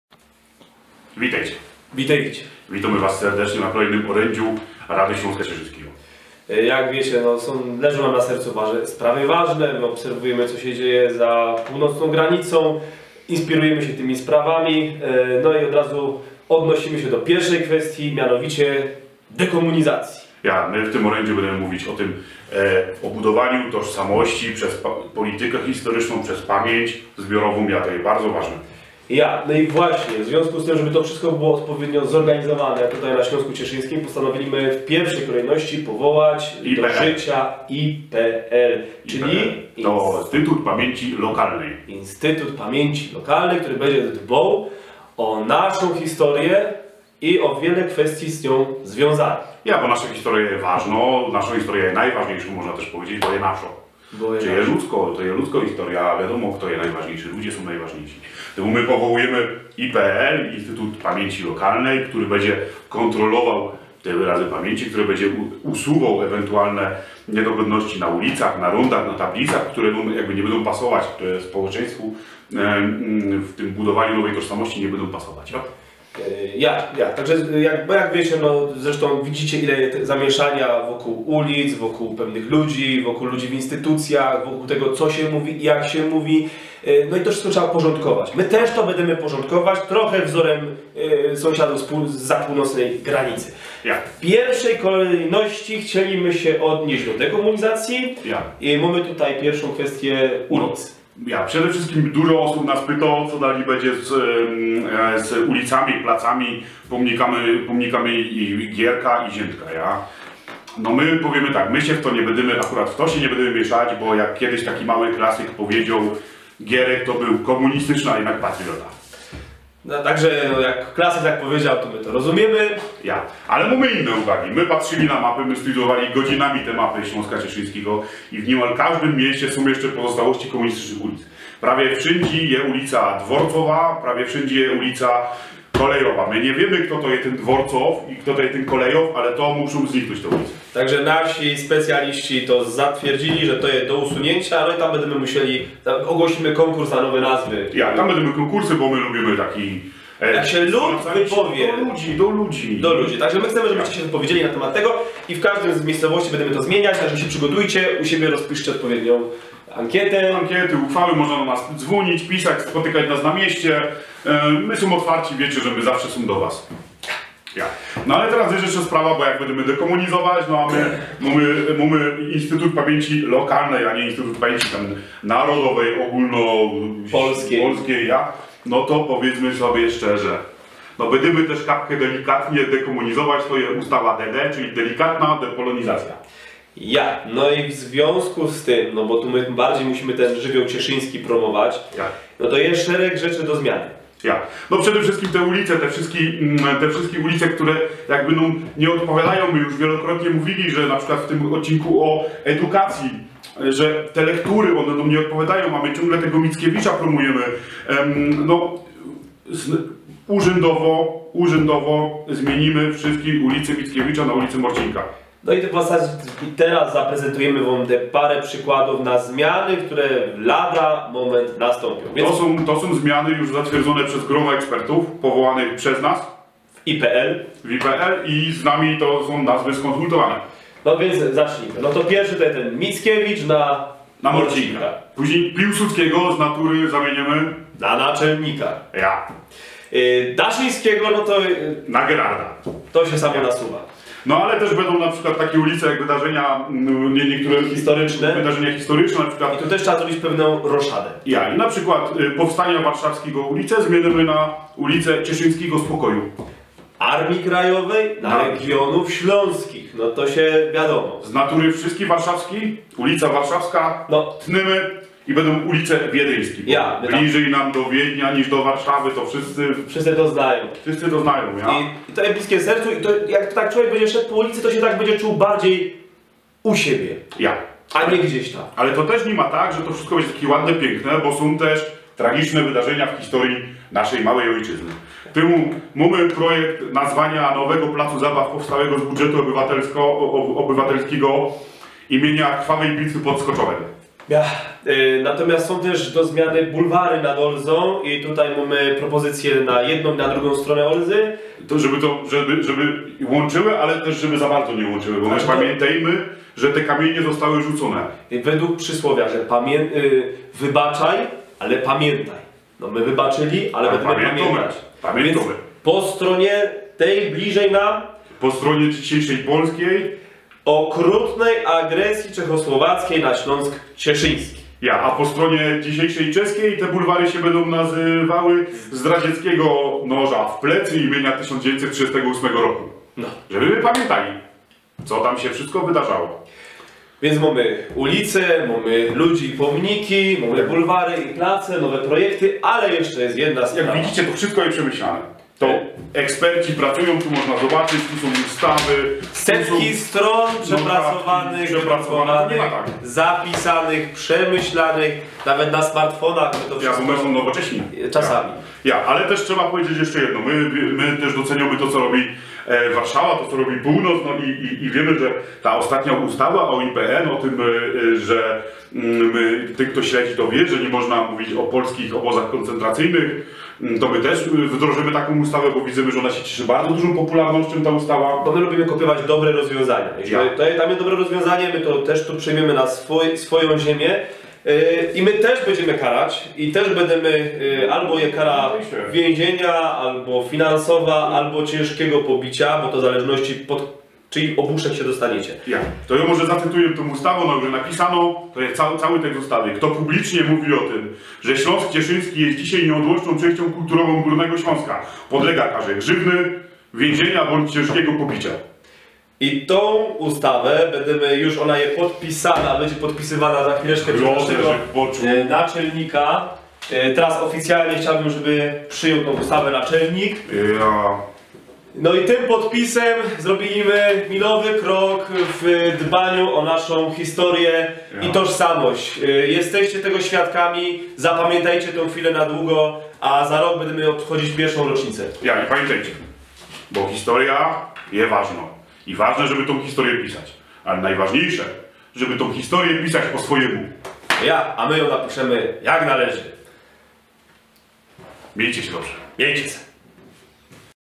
Rada Śląska Cieszyńskiego #19: Instytut Pamięci Lokalnej i polityka delikatnej depolonizacji
Każda społeczność potrzebuje historii. A najlepiej, jak tę historią piszemy po swojemu. Dlatego powołujemy Instytut Pamięci Lokalnej i formujemy cieszyńską politykę historyczną!